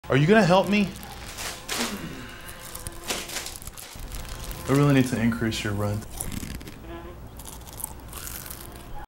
How to help a roommate clean up after a party like a boss.
Gil handles clean up like a boss.

Performed by Gary Ploski.